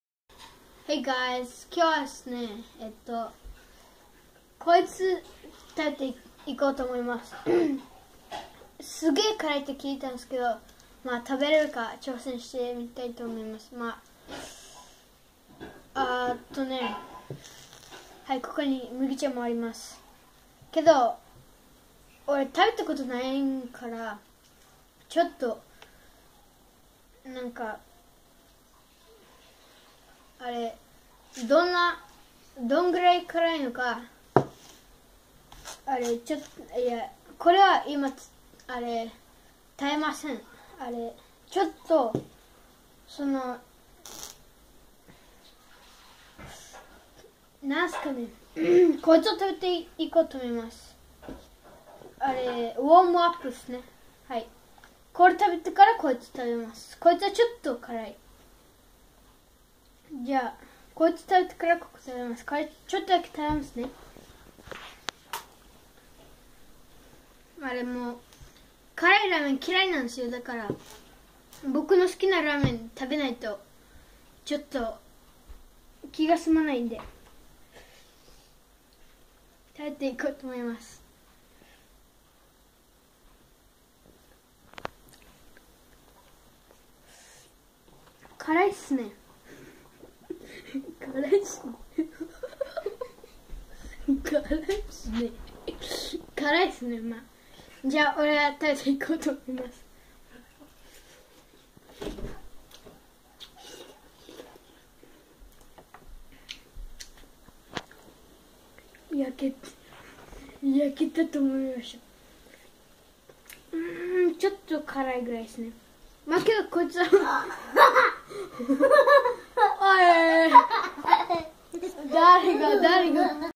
I ate a spicy Korean noodle part 1(Japanese)
Is a spicy noodle ( I was going to die)